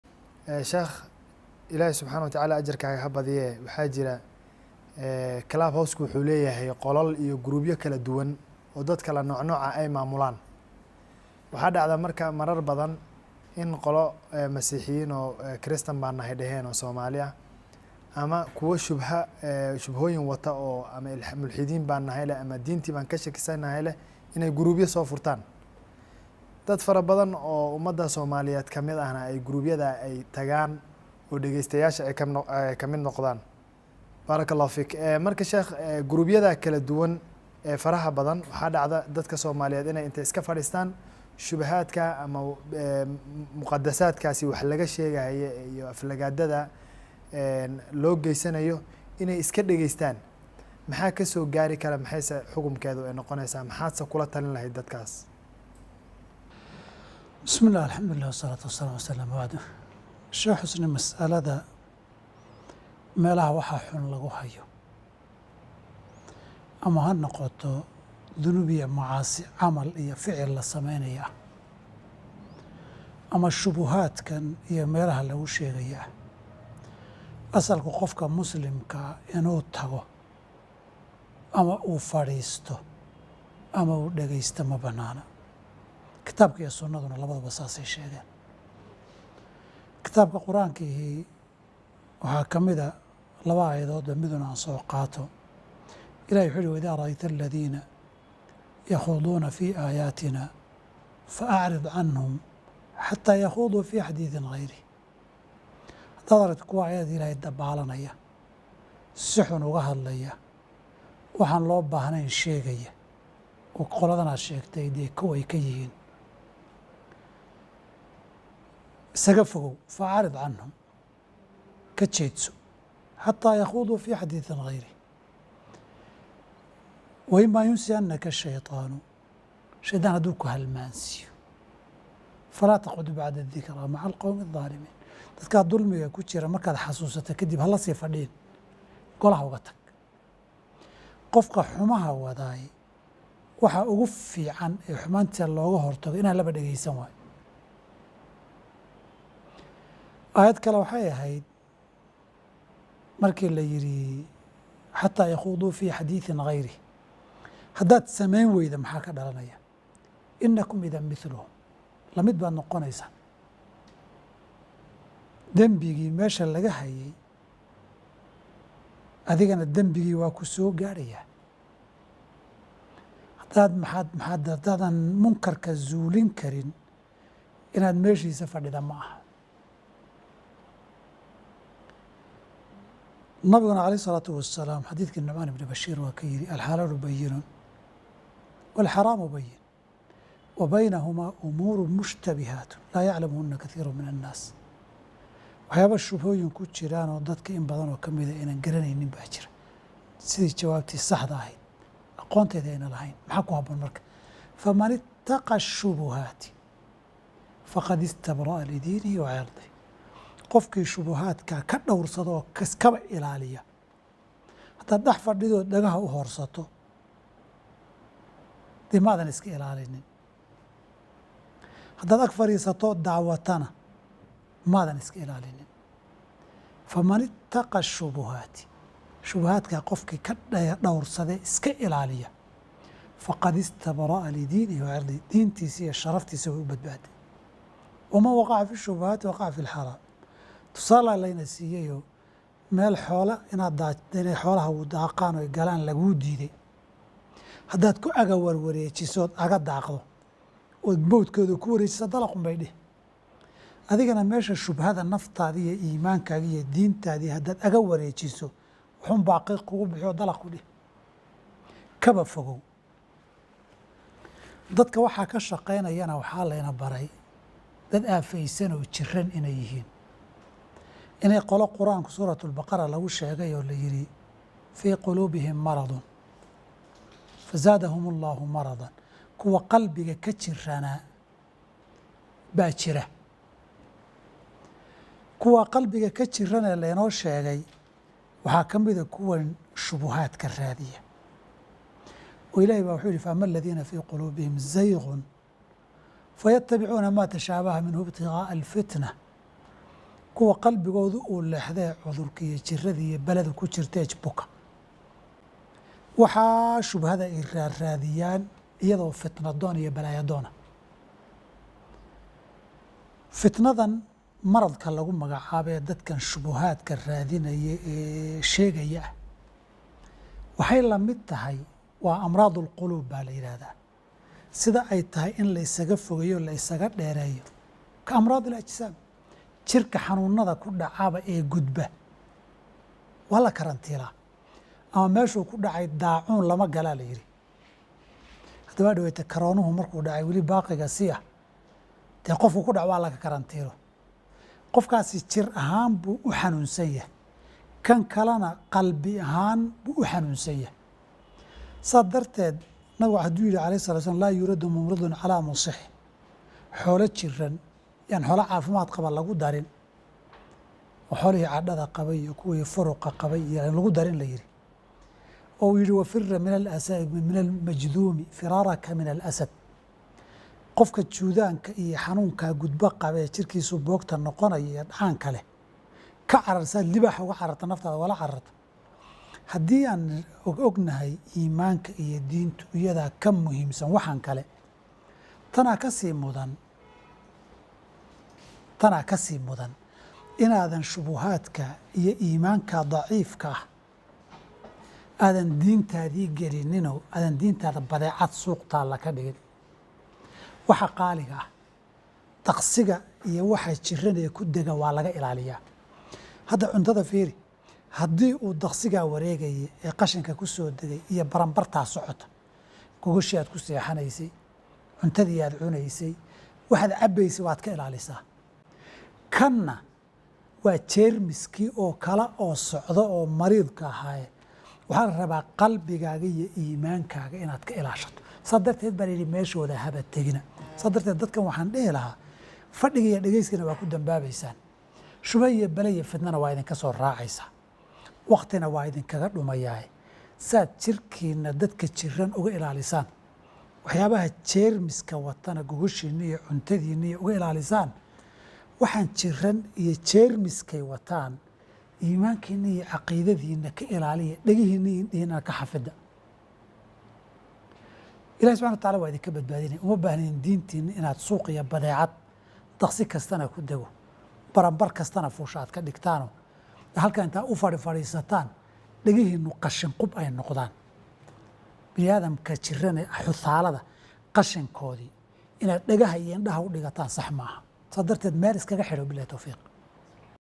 XUKUNKA SHAREECADA EE LA JOOGGA MURTADDIINTA || Dr Sh Maxamuud Shible
kuwa iska dhex fadhiya qolalka gaalada ilaahay haka baqaan

‎ { وَتَعَاوَنُوا عَلَى الْبِرِّ وَالتَّقْوَىٰ وَلَا تَعَاوَنُوا عَلَى الْإِثْمِ وَالْعُدْوَانِ وَاتَّقُوا اللَّهَ إِنَّ اللَّهَ شَدِيدُ الْعِقَابِ

Isu kaalmeeysta samaha iyo Dhawrsiga (Alle ka cabsiga hana Isugu Kaalmaynina dambi iyo Colnimo, (Xadgudub) kana Dhawrsada allaah Ilaahay way darantahay ciqaabtiisu